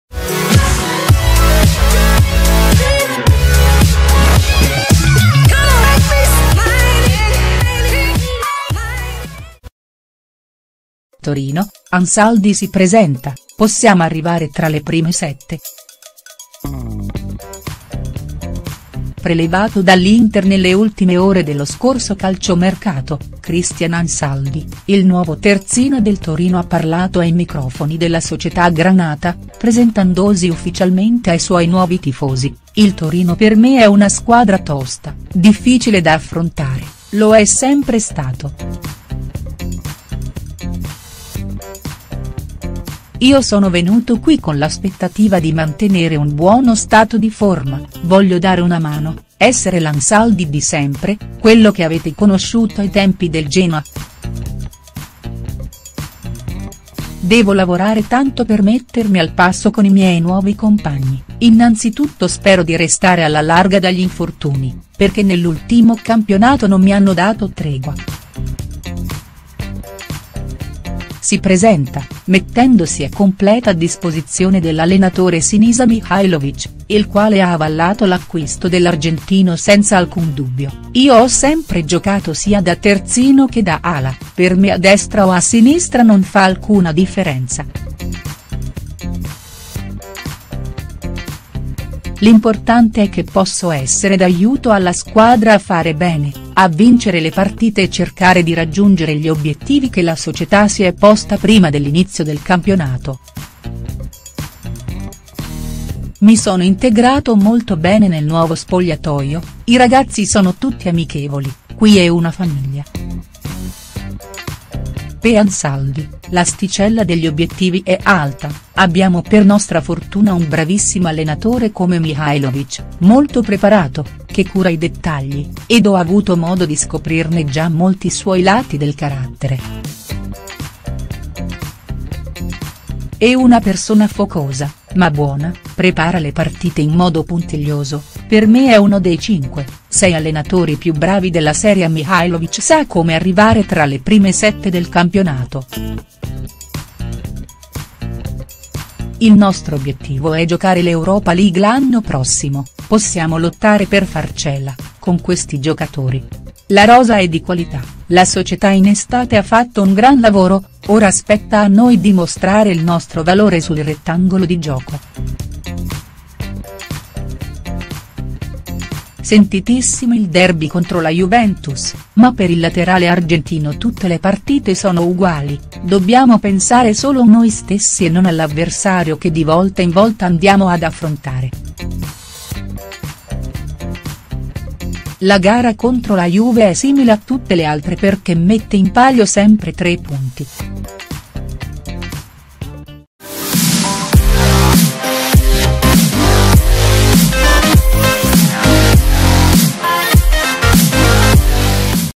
Torino, Ansaldi si presenta: "Possiamo arrivare tra le prime sette".HD
Motivazioni alte, l'ex Inter vuole l'Europa League, sfuggita di mano quando era all'Inter.